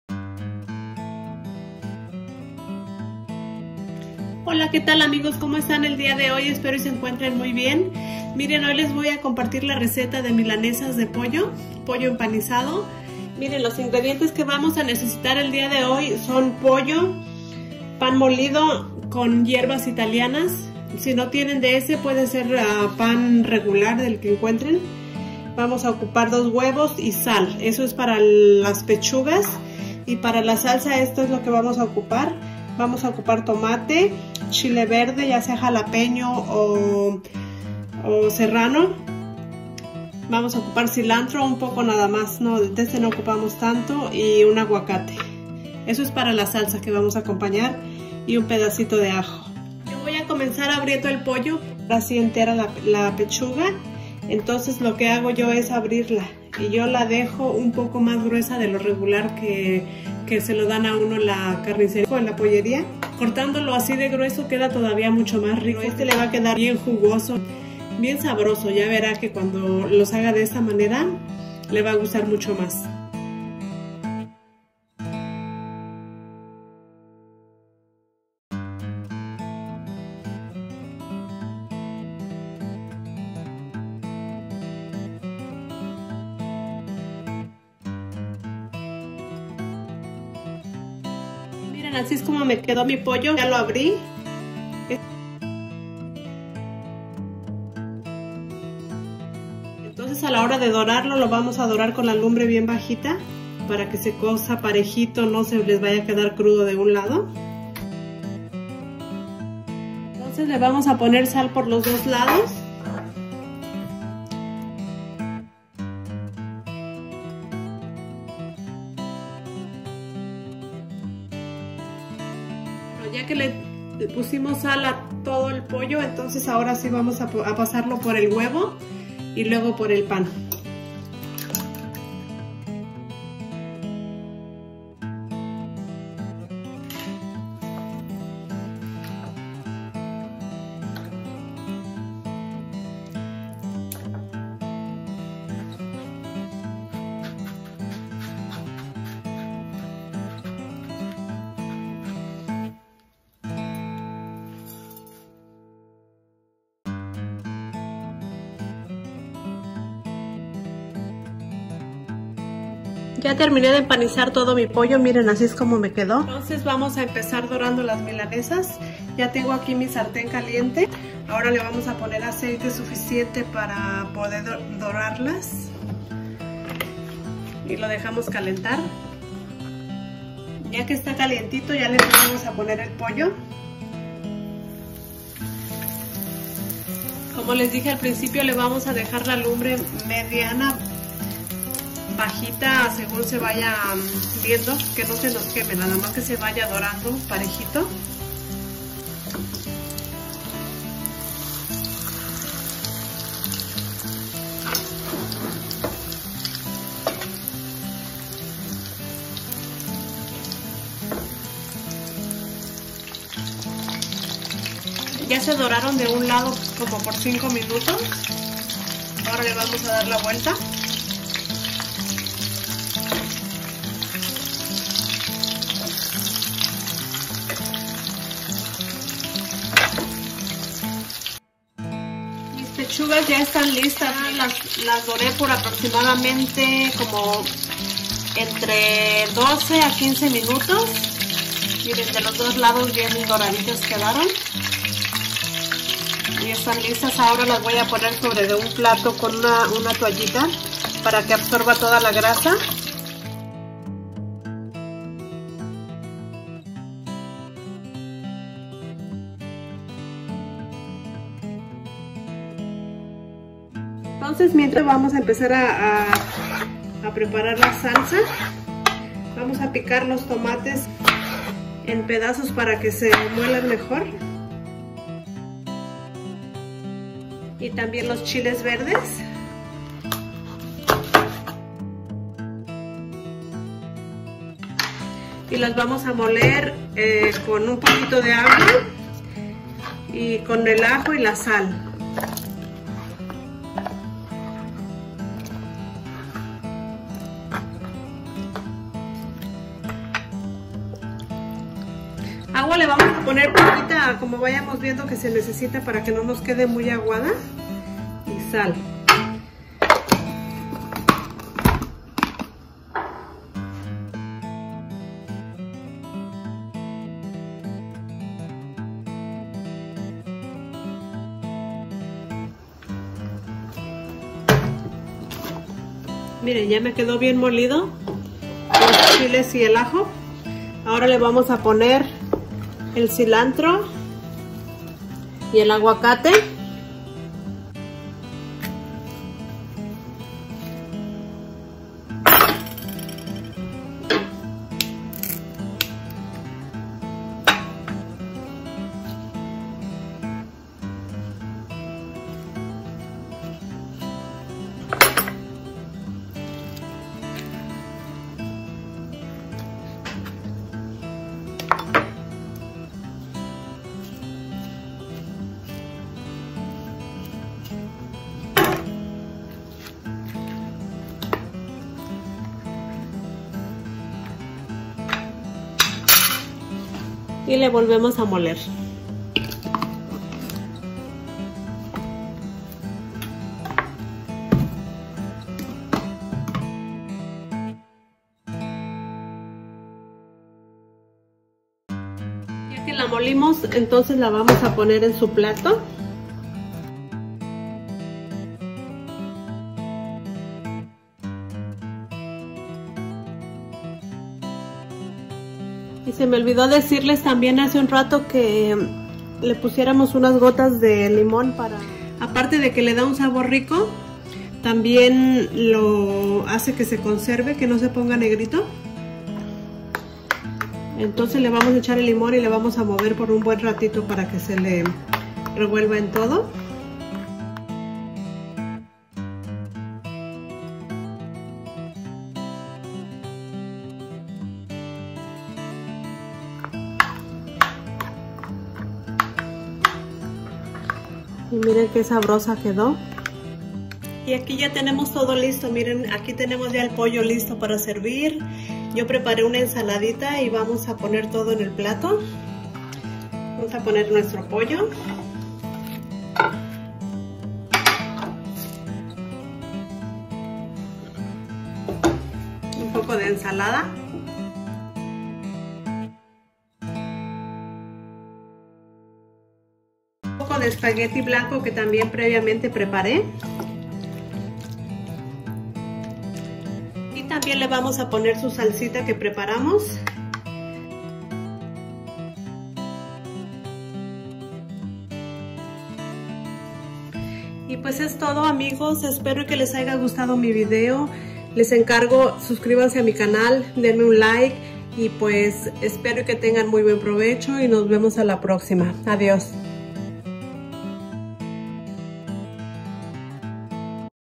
MILANESAS DE POLLO CASERAS || PECHUGAS EMPANIZADAS SUAVECITAS
Pechugas empanizadas o milanesas de pollo. Una receta deliciosa, facil y nutritiva que a toda la familia le encanta con su salsita verde o roja un spaghetti o un arrocito es una exelente comida para disfrutar en familia.